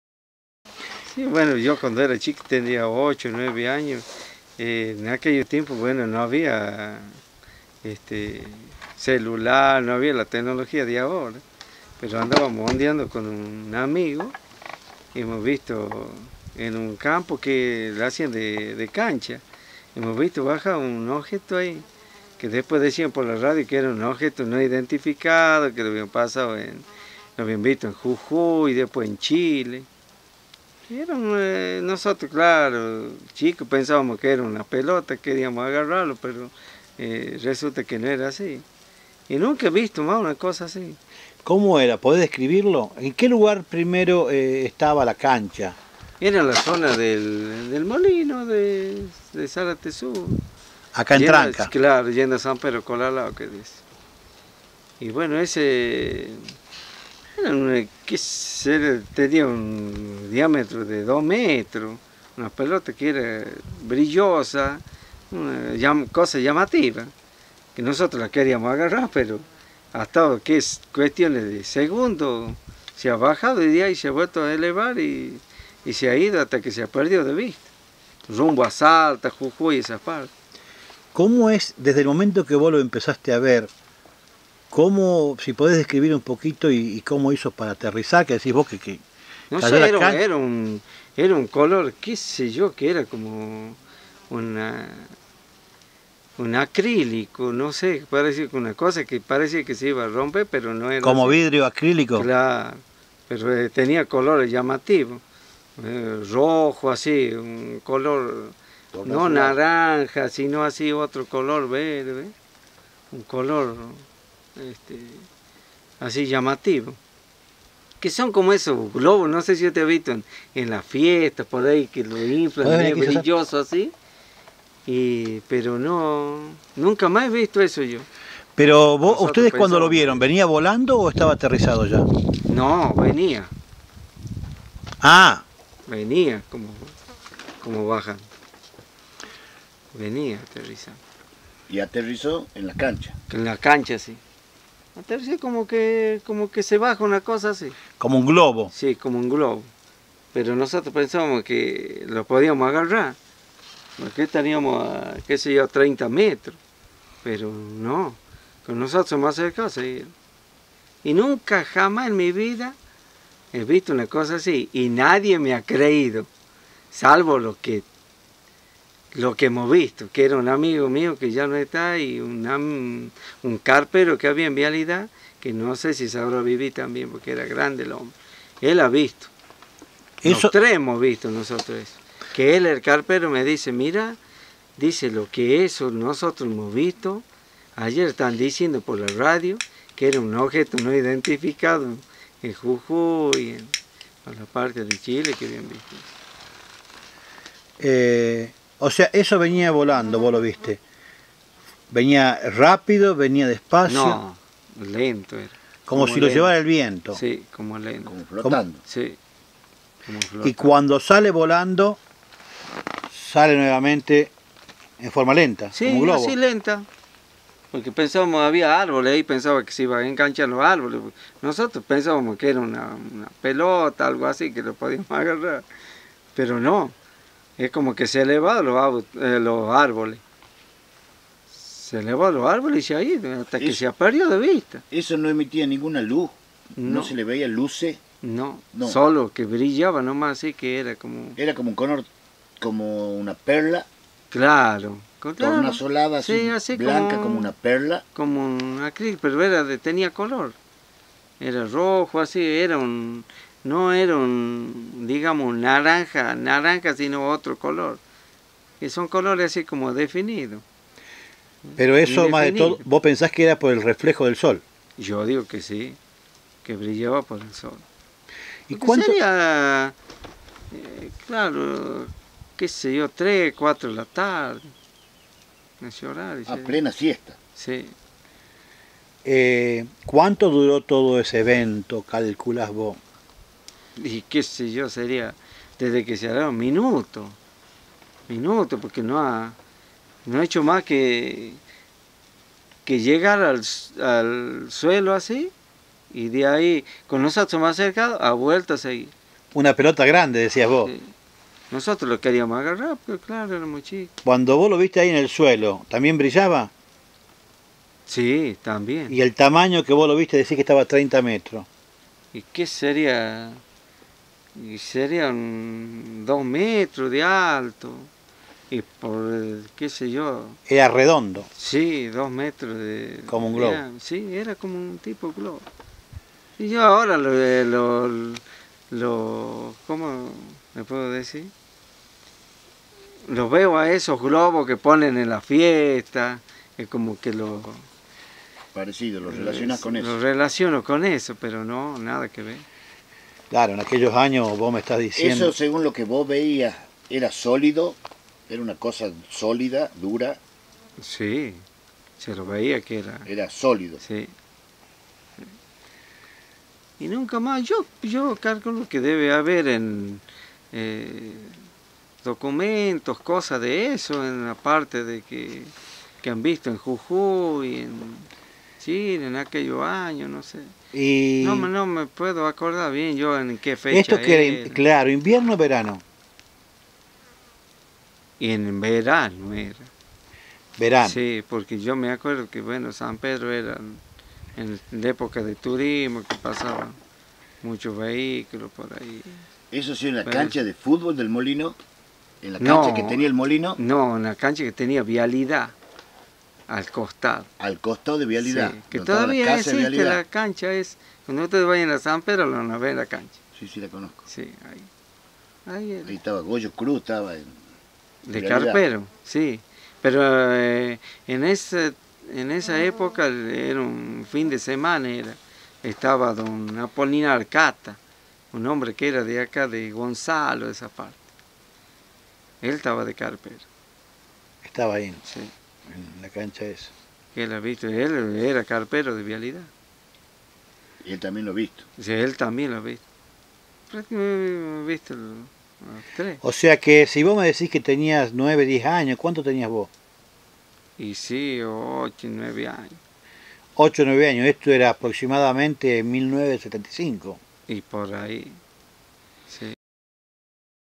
HISTORIA DE OVNIS Recuerdos de Pepe Cossa
HISTORIA DE OVNIS
Testigos : 3
Fecha : Verano del 1975-77
Lugar :Zarate Sur, unos 10km antes de llegar a San Pedro de Colalao -Tucumán

Hoy compartimos el testimonio de Francisco Cossa ,donde nos cuenta de una extraña experiencia vivida cuando era niño, junto a un amigo de su edad y una persona mayor, en una tarde de verano de 1975,observaron el descenso de una esfera de unos 2m de diámetro, que parecía de acrílico, y luego de unos momentos se elevó emitiendo destellos muy impactantes.